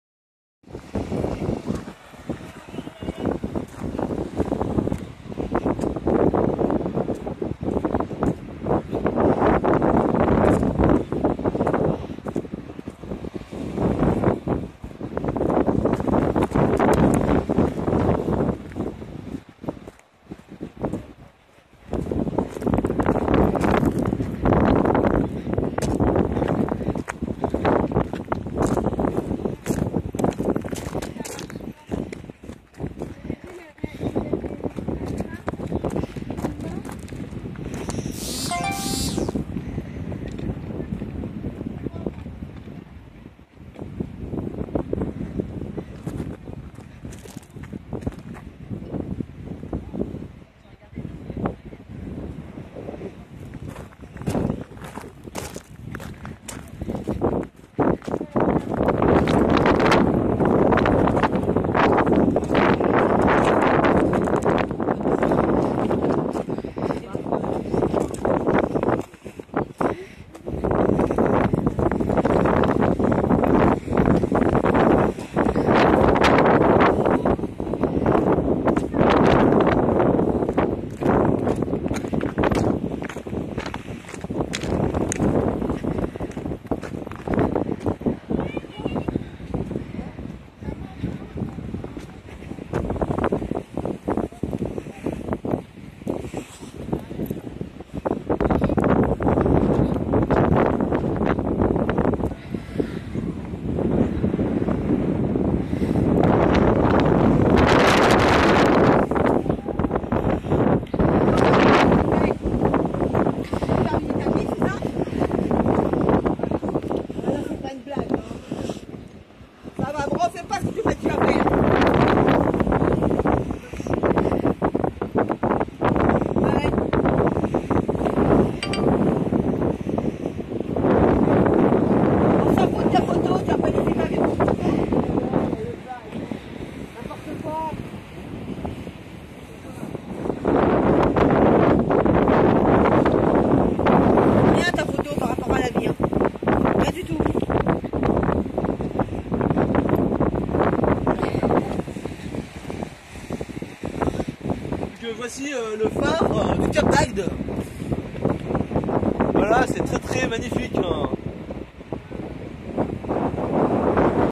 balade au Cape d'Hague 🌊 #24
J ai fait une petite balade sympa au Cap d' Hague. les paysage son tellemen magnifique que je tente de refaire bien sur. Ha par contre dsl pour la sonnerie des notif. Voila j espere que cette vidéo vous aura plus n hésite pas à mettre un pouce bleu 👍 partage et commente a fond